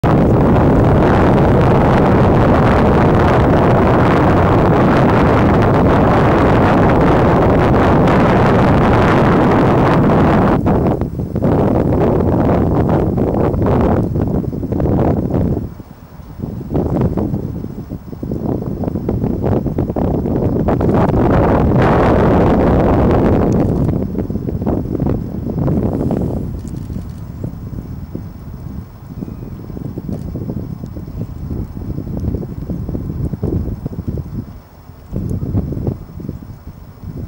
Great gray owl, Newport, NH. 3/9/17 in snow squall.
This great gray owl was hunting mice and voles along a large field in Newport NH when a snow squall with 25 mph winds arose. TURN DOWN VOLUME AS WIND IS TERRIFIC.